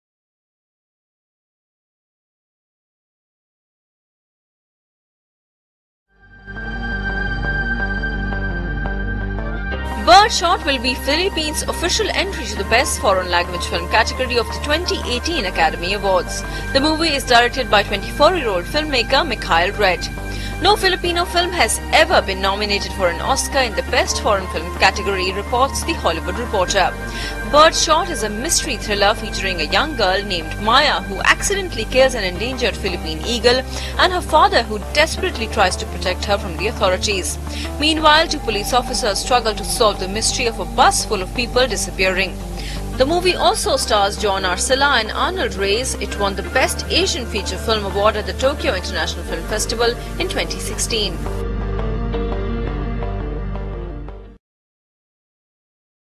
'Birdshot' is Philippines' official entry to the Best Foreign Language Film category at Oscars
'Birdshot' will be the Philippines' official entry to the Best Foreign Language Film category of the 2018 Academy Awards. The movie is directed by 24-year-old filmmaker Mikhail Red. No Filipino film has ever been nominated for an Oscar in the best foreign-language film category, reports The Hollywood Reporter. 'Birdshot' is a mystery thriller, featuring a young girl named Maya (Mary Joy Apostol), who accidentally kills an endangered Philippine eagle, and her father (Ku Aquino), who desperately tries to protect her from the authorities. Meanwhile, two police officers struggle to solve the mystery of a bus full of people disappearing. The movie also stars John Arcilla and Arnold Reyes. It won the Best Asian Future Film award at the Tokyo International Film Festival in 2016